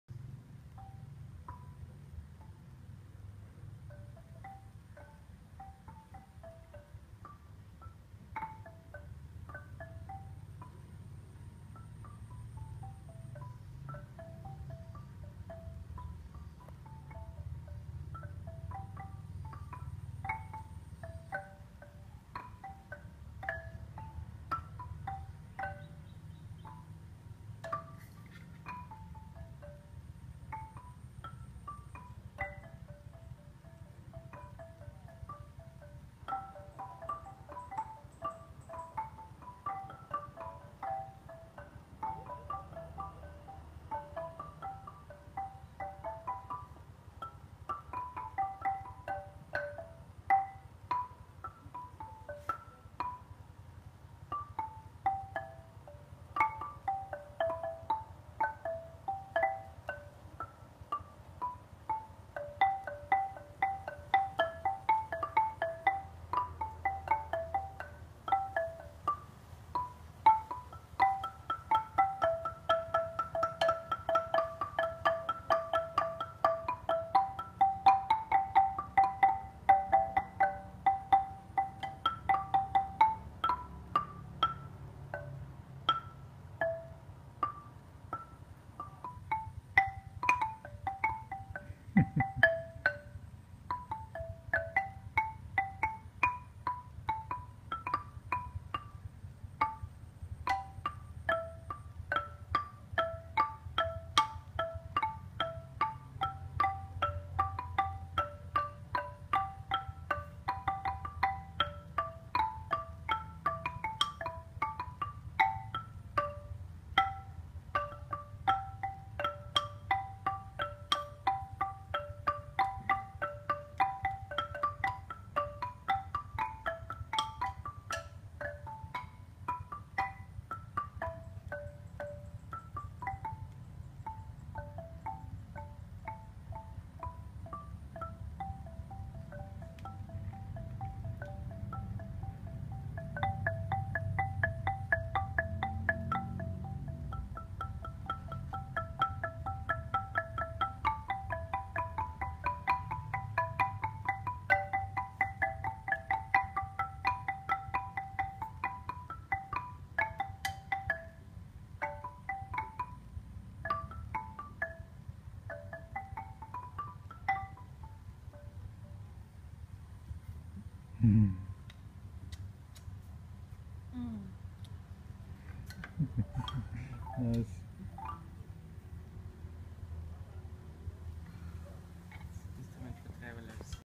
Heart Bridge Dialog 5 - Herzbrückenspiel 5 - Bali, Tegalalang, tropical Klangwerk studio 2017
it's fun...it's love increasing... its without presuppositions... playful...  joy giving... connecting heart to heart... it's simple but magic... it works beyond any cultural and ethnical imprint: get musical without having to be a musician!
A basic tool for nonverbal communication.